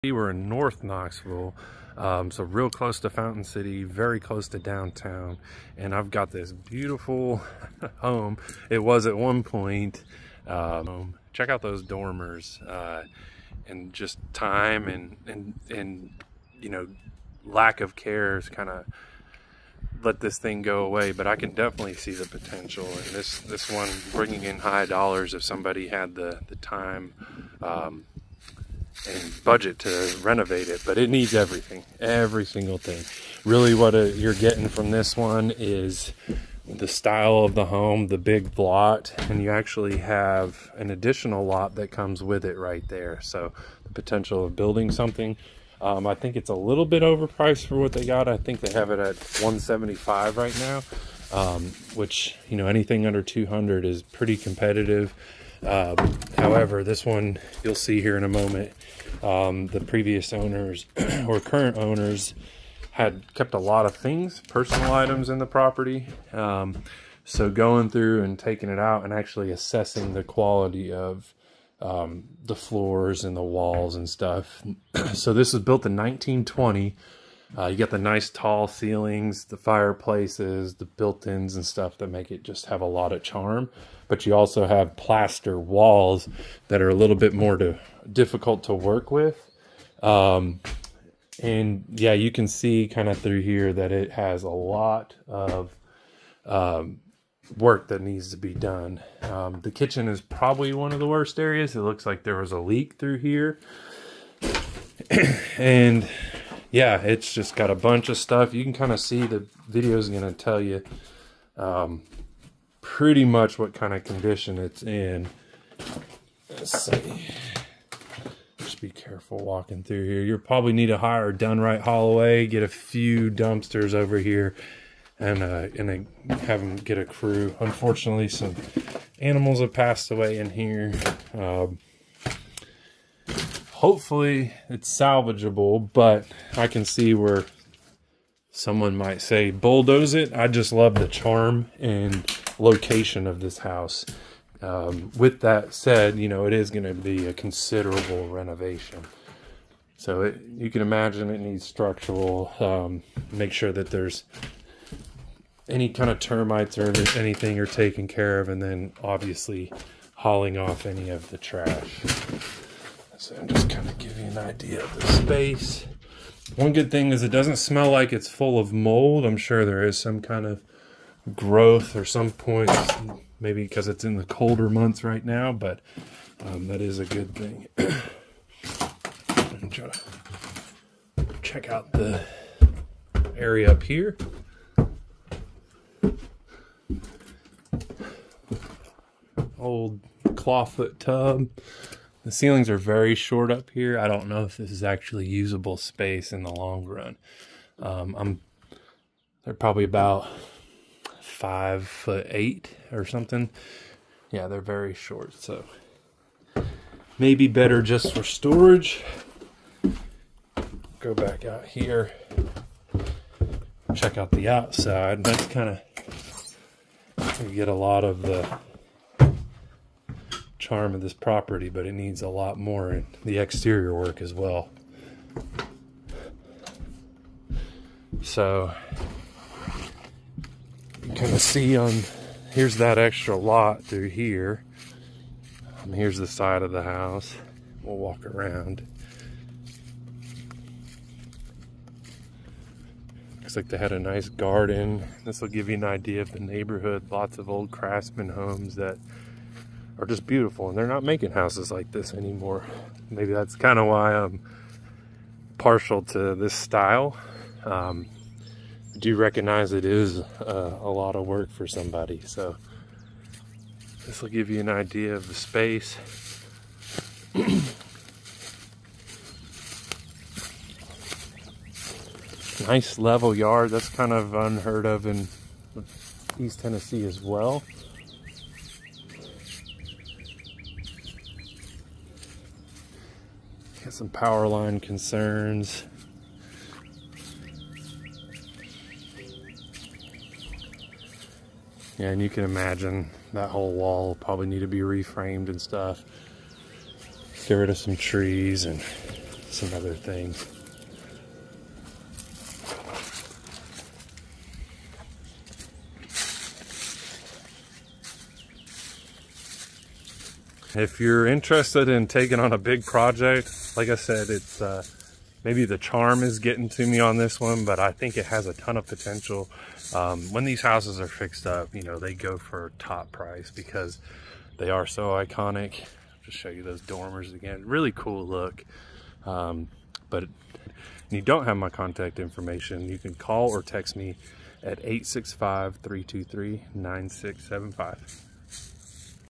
Can this craftsman be brought back to life? Hoarder house in Knoxville, TN
I am a sucker for an old Craftsman style home. This once was a gorgeous property that fell into disrepair and used as a hoarder home. What do you think, can it be saved?